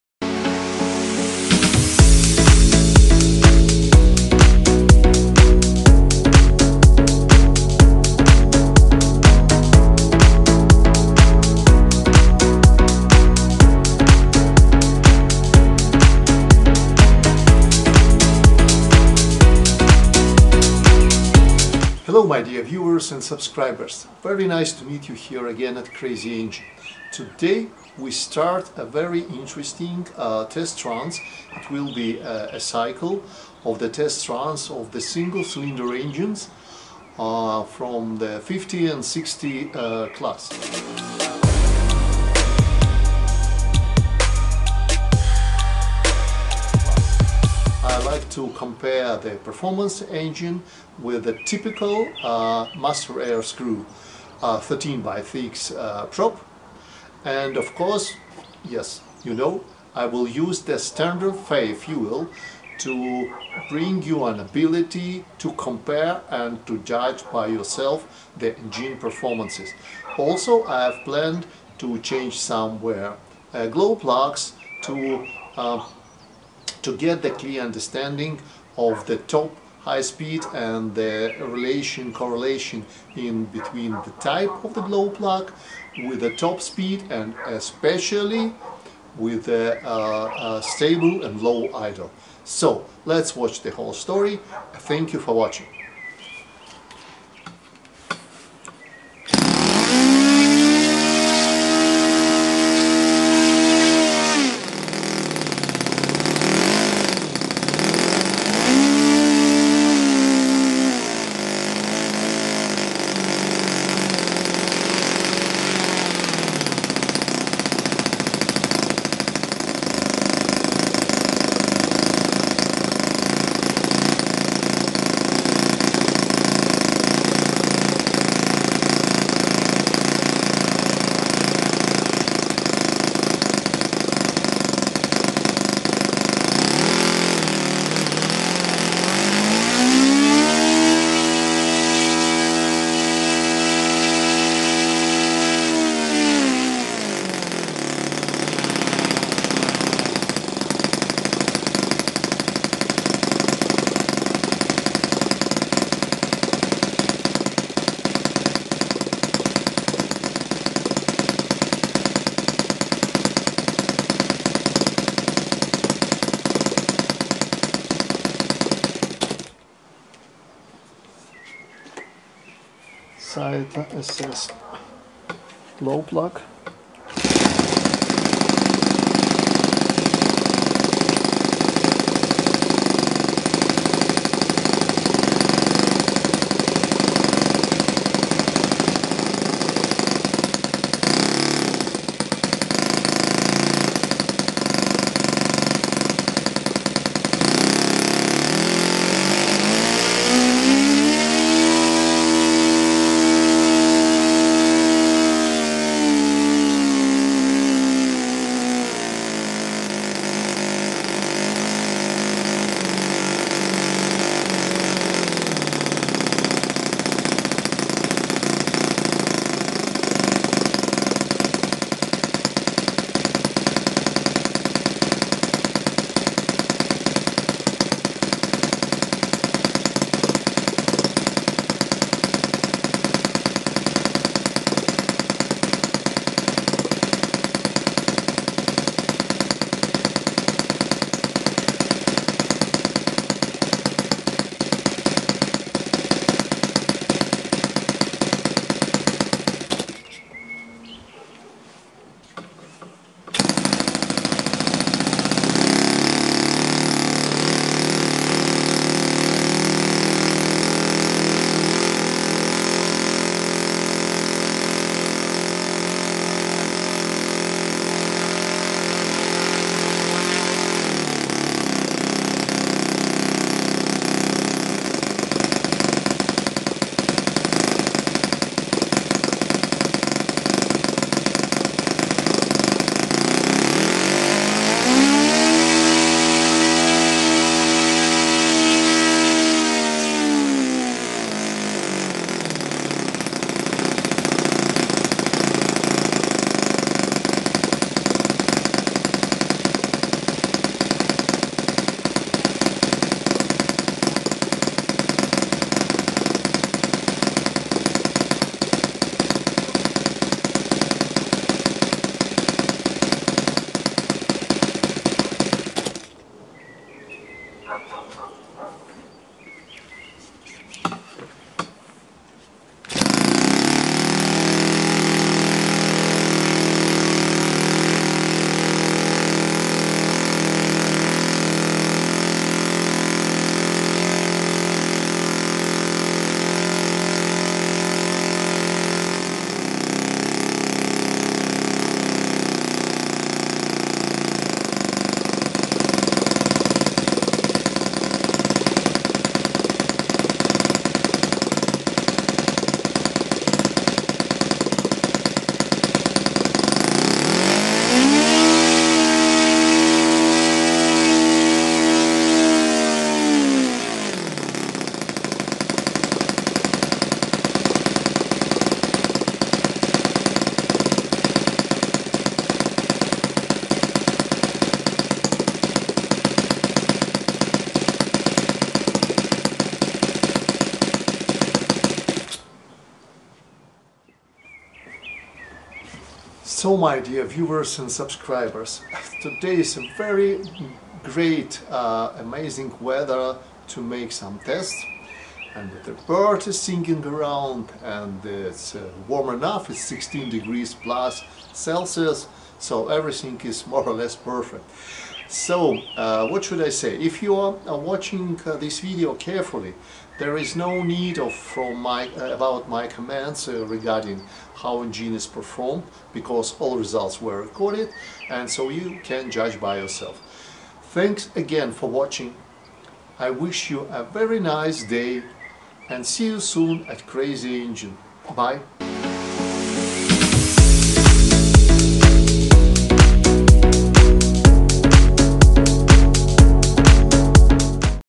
Saito FA 56 GK comparison test run
This is the first video story from a planned cycle of test runs with the single cylinder engines from 50-60th class. All 11 engines will be consequentially tested on the bench with the standard MAS 13x6 prop and on  FAI standard fuel. This will give an ability to compare engines performance in general.  Also, at least, 4 types of the glow plugs will be used in this test runs cycle.
This is the first comparison test run with Saito FA 56 Golden Knight.
Let's watch the whole story.